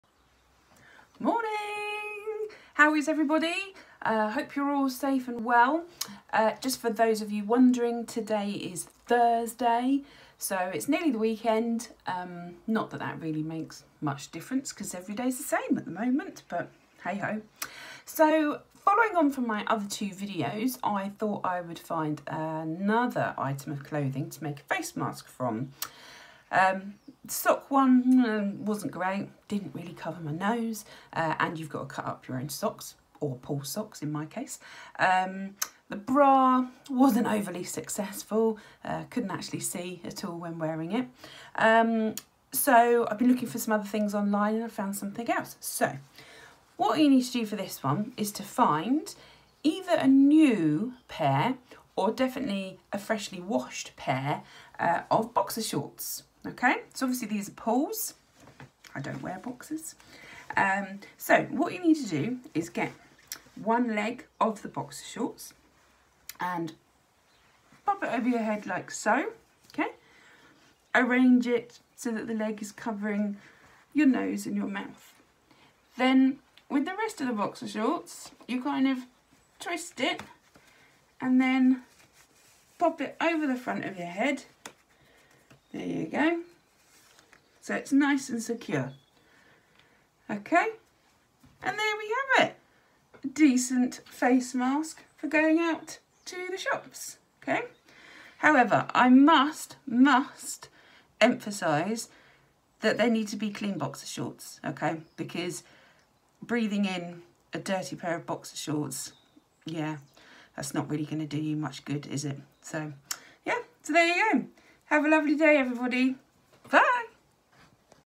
Home made face mask using boxer shorts 😊🩲
This video is for humour only and is in no way a serious way to make a face mask